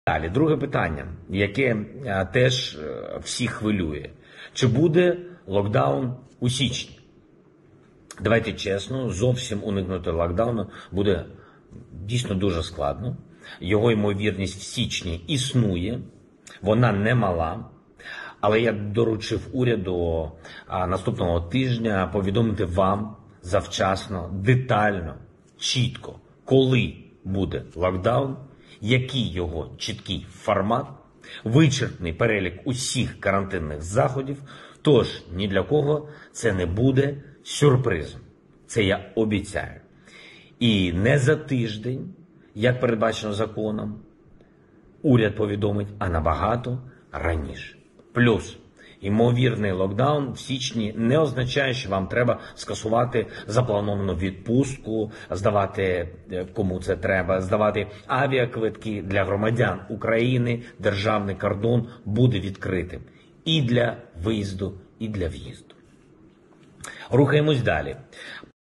Локдауна в декабре не будет, заявил Владимир Зеленский.
Также гарант назвал основные программы поддержки населения и бизнеса в условиях пандемии, которые начнут функционировать уже скоро.